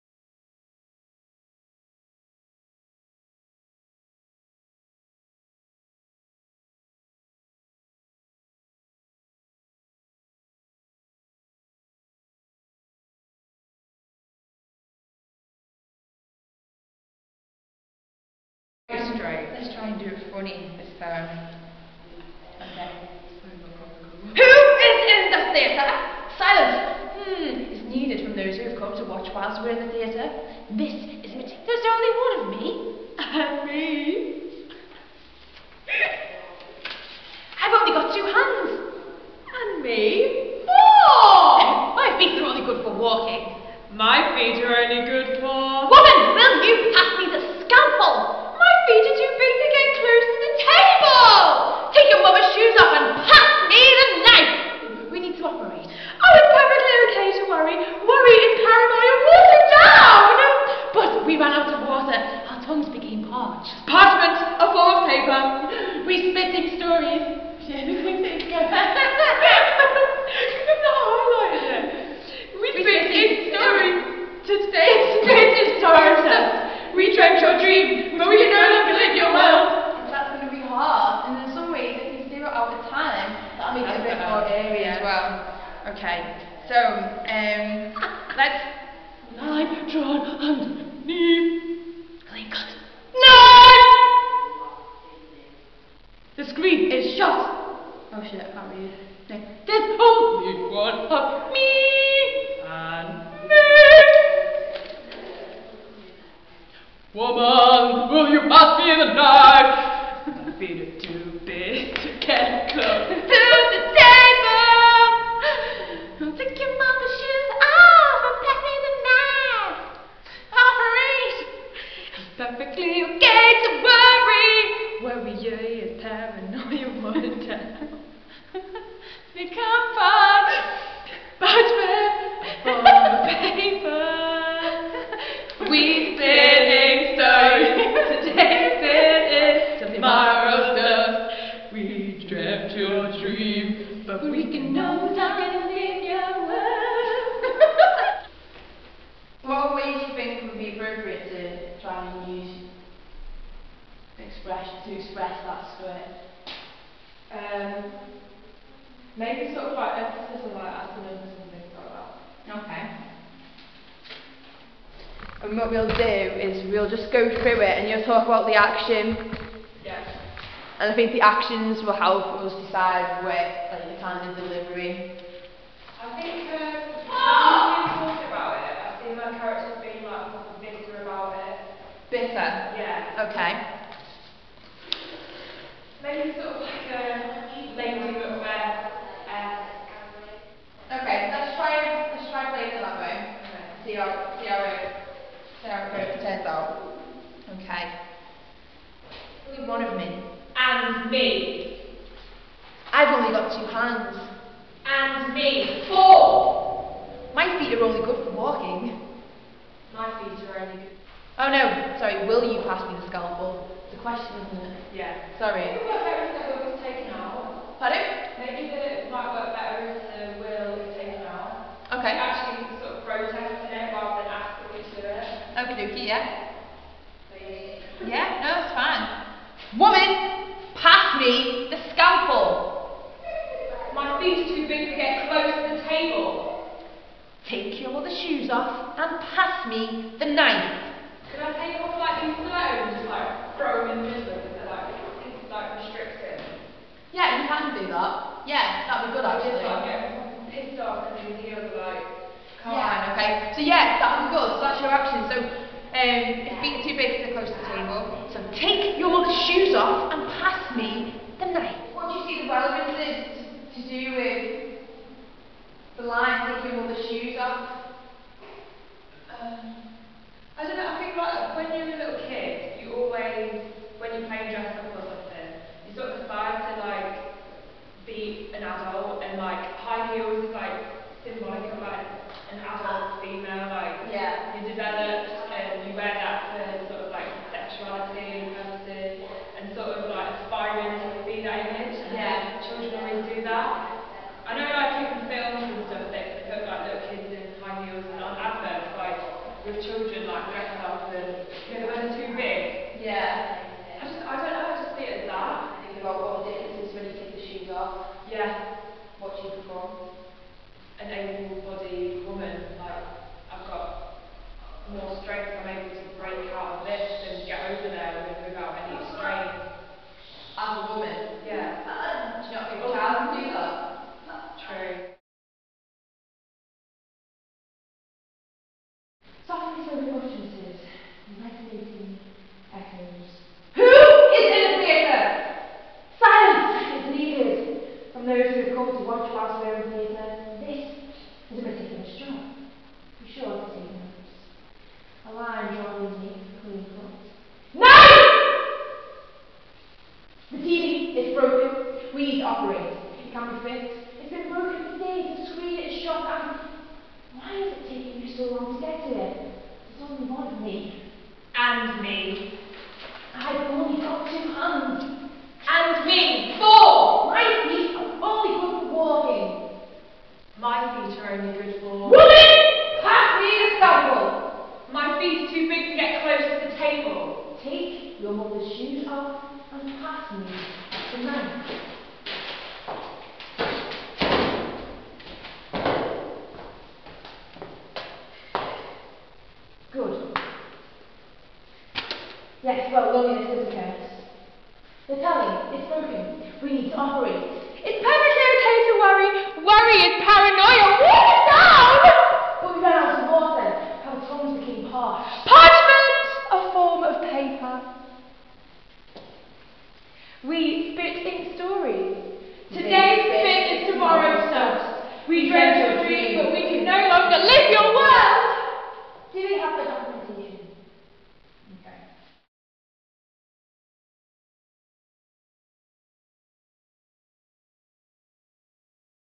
SAN WORKSHOP 7.2
Georgia and Carly begin working through the actions to Georgia's dialogue.